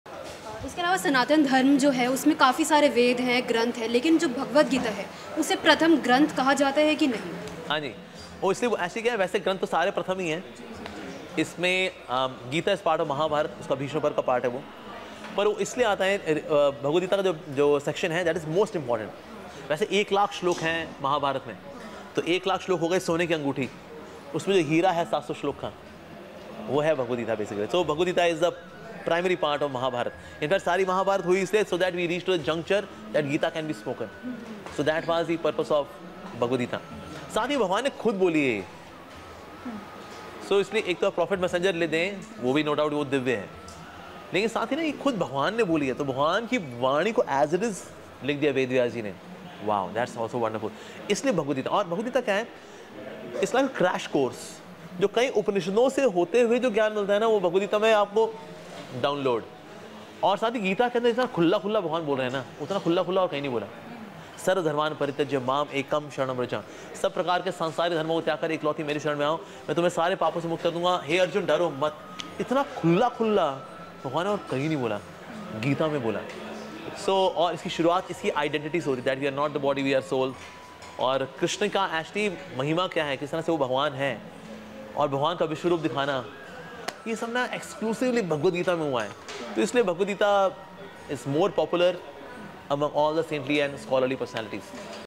क्या भगवत गीता प्रथम ग्रंथ है? | Shrimad Bhagawad Geeta | HG Amogh Lila Prabhu
क्या भगवत गीता प्रथम ग्रंथ है? | Shrimad Bhagawad Geeta

यदि आप इस पर आर्थिक मदद करना चाहते हैं तो कृपया इस लिंक का उपयोग करें

Reported By :
Cameraman :
Edited By :